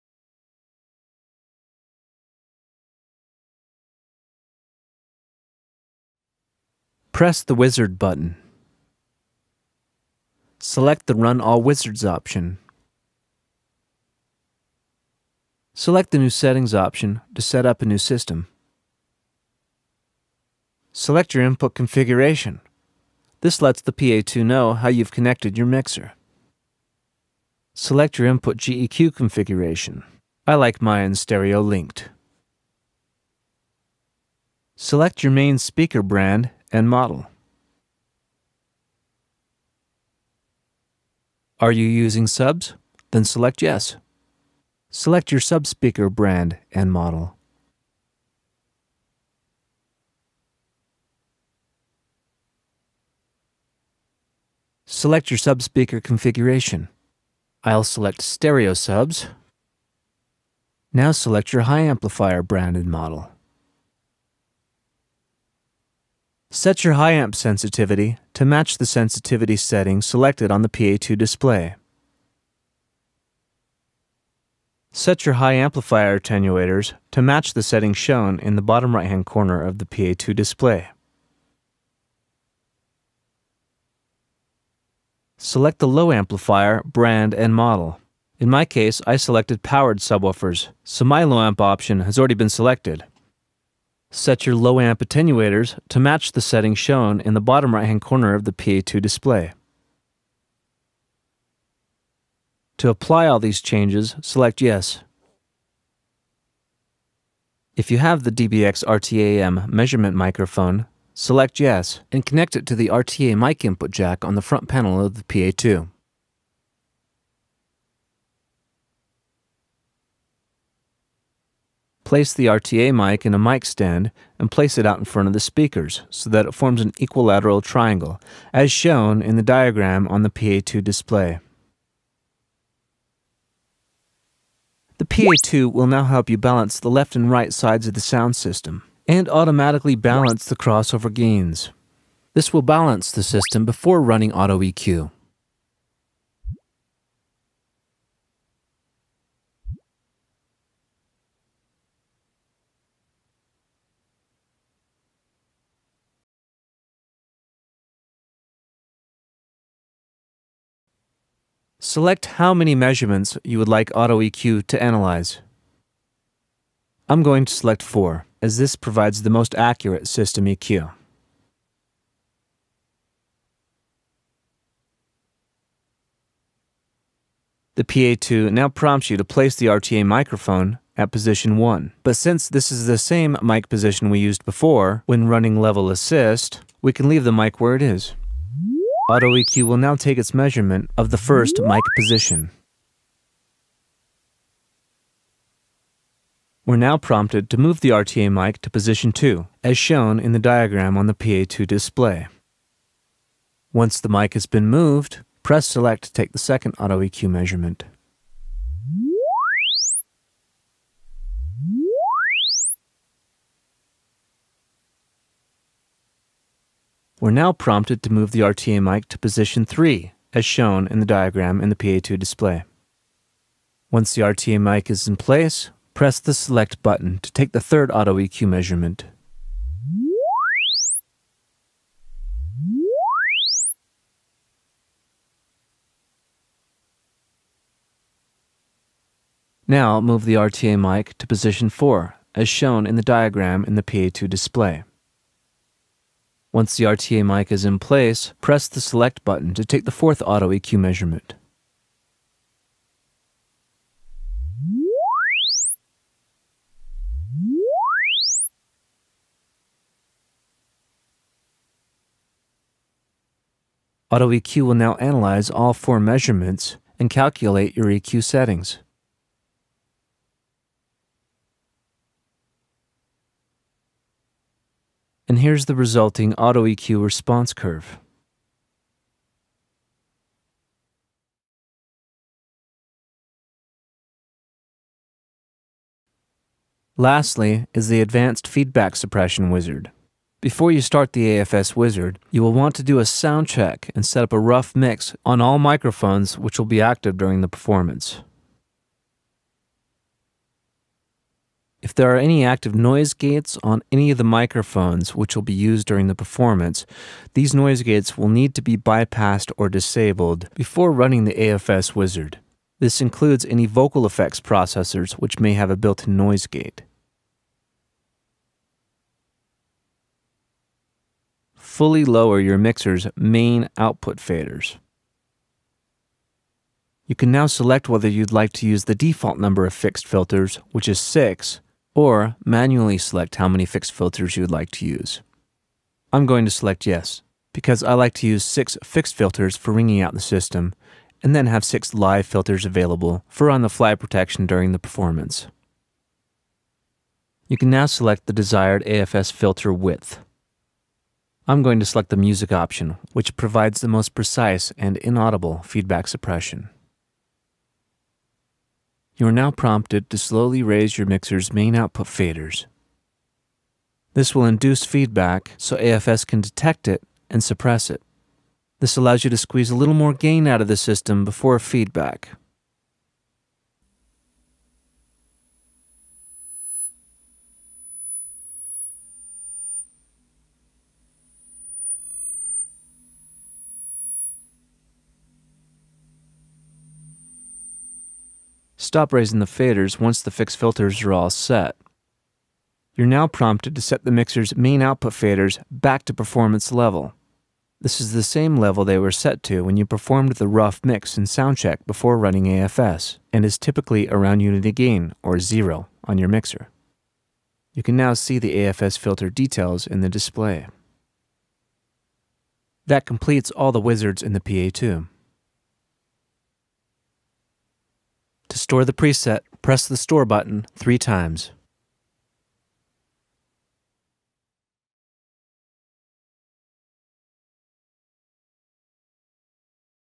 dbx DriveRack® PA2 Wizard Setup Video
This video details the process for running the dbx DriveRack PA2 Wizards. The DriveRack PA2's Wizards walk you through the configuration process with simple, step-by-step instructions, making it easy to configure the PA2 for your amplifiers, speakers and venue.

It covers:
"Setup Wizard" functions - which consist of Input Setup, GEQ (Graphic EQ) Setup and Speaker/Amp Setup.

"Input Setup" - allows you to select your input configuration between mono and stereo.

"GEQ (Graphic EQ) Setup" - allows you to select your GEQ configuration between dual mono and stereo linked.

"Speaker/Amp Setup" - allows you to select your speakers and/or amplifiers so the PA2 can optimize its settings for your particular system components.

"AutoEQ/Level Assist Wizard" - helps you balance the left/right speaker levels, the low/mid/high speaker levels (for 2-way and 3-way systems) and automatically equalizes the speakers to the current room environment.

"AFS (Advanced Feedback Suppression) Wizard" - walks you through the process of ringing out your system to provide higher system gain before feedback.